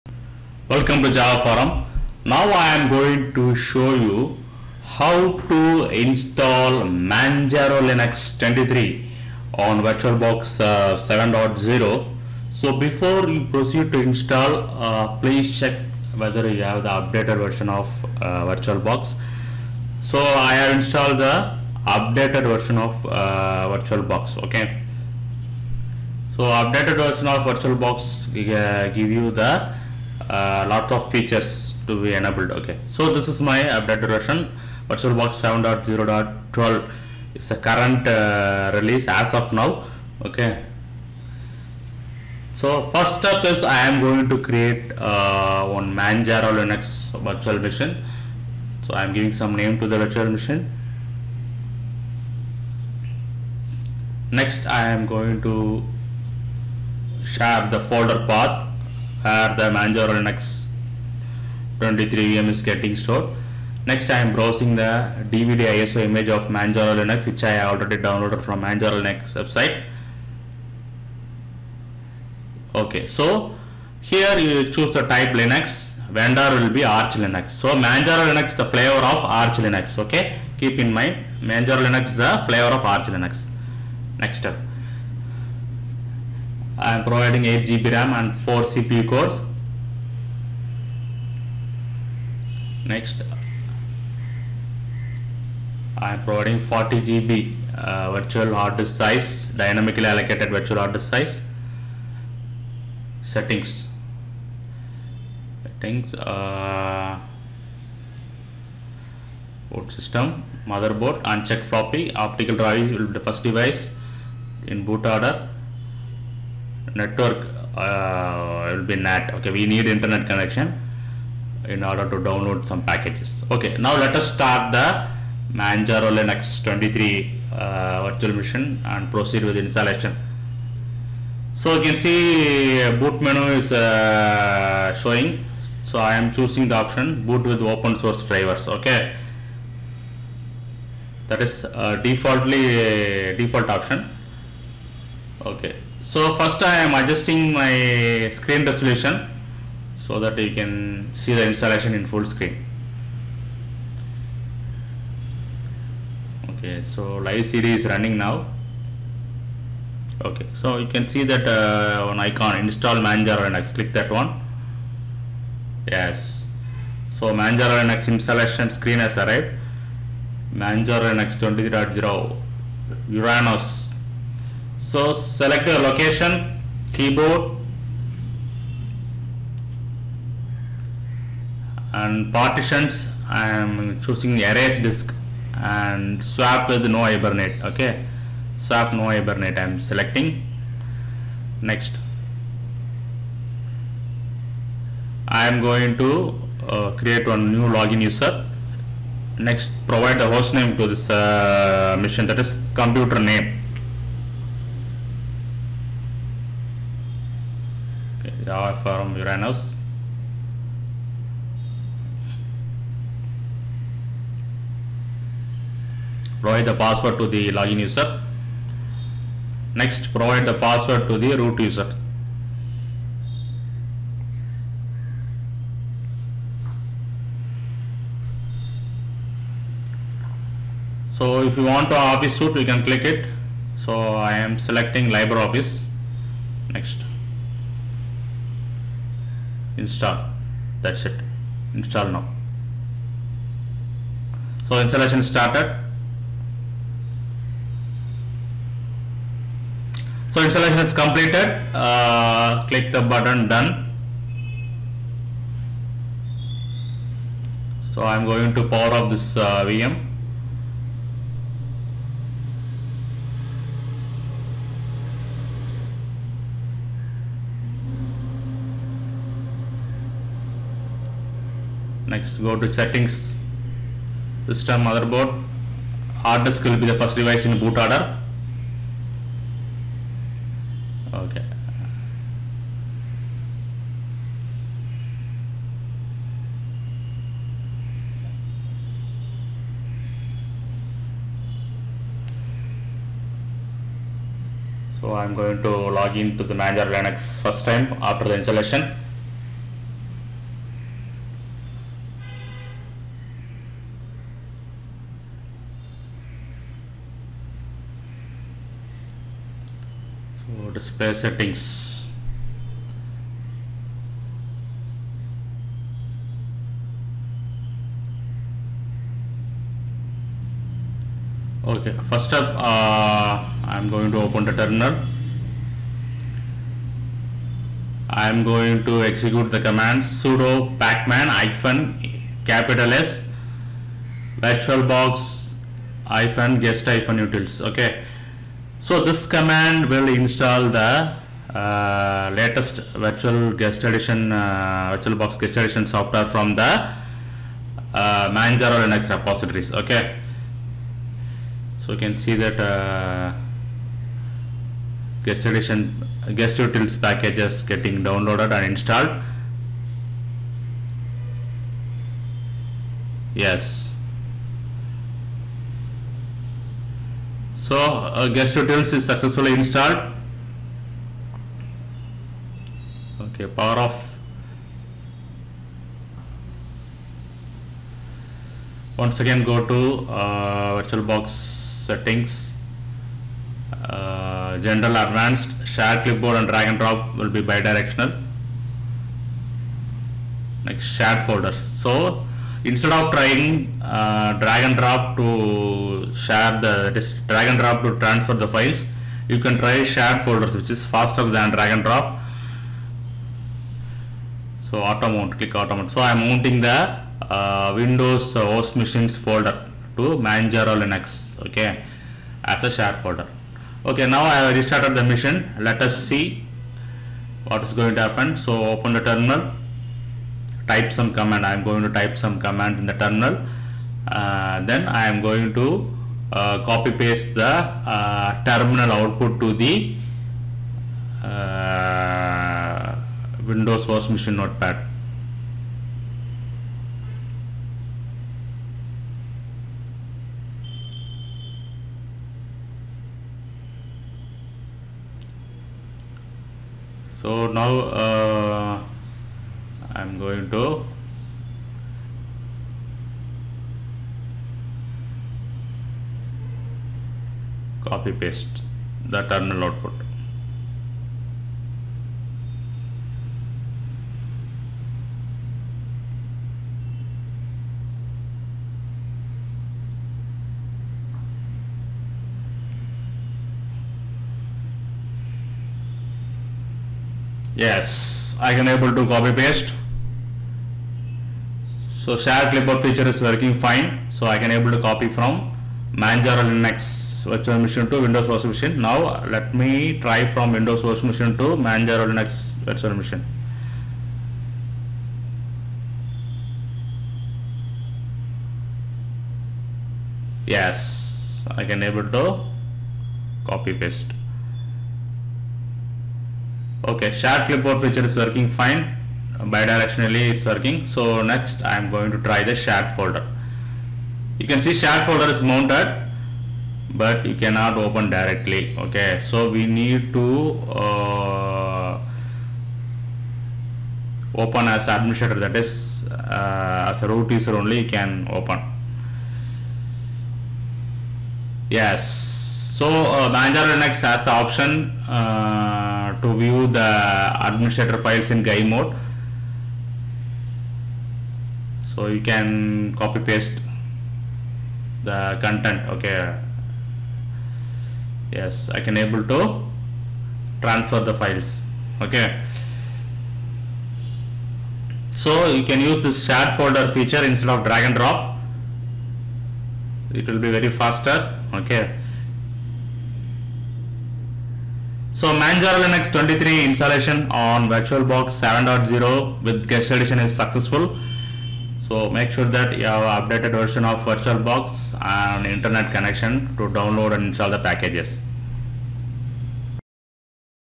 Manjaro Linux 23.0 Installation on VirtualBox 7.0 with Guest Additions
In this video we are going to see how to install Manjaro Linux 23.0 on VirtualBox 7.0 and install Guest Additions.
Manjaro Linux 23 is the latest major release. In this video we shown
1. how to install dependencies for guest additions.
2. download and install guest additions from internet.
3. how to enable Shared Clipboard.
4. how to access Shared Folder.

Popular Linux Distributions (2023) with VirtualBox 7 Guest Additions